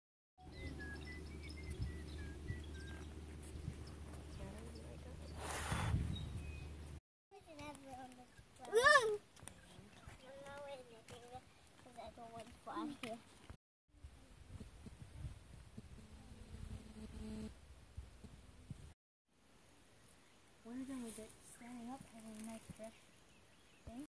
day trip on the river
A seven year old and her family spend a day on the river together.